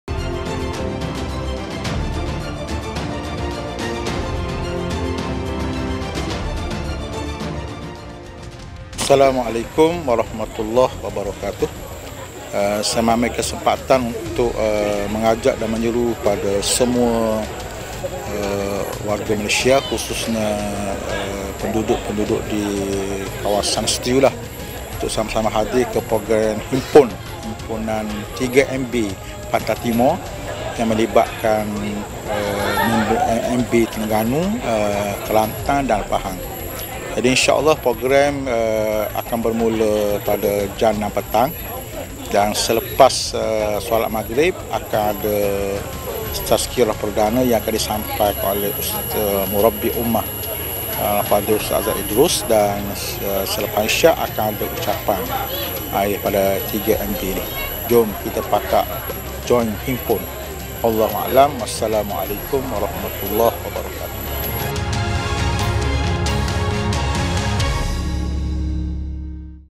Seruan Jom Hadir ke Program HIMPUN
Seruan jom hadir ke program HIMPUN oleh YB Ustaz Shaharizukirnain Abd Kadir, Ahli Parlimen Setiu.

Terengganu, Kelantan dan Pahang bakal menjadi simbolik perpaduan negeri Pantai Timur.

Jangan lepaskan peluang. Ayuh hadir HIMPUN!

22 OGOS 2019 | KHAMIS | STADIUM TERTUTUP KOMPLEKS SUKAN NEGERI, KUALA NERUS | 6.00 PTG - 11.00 MLM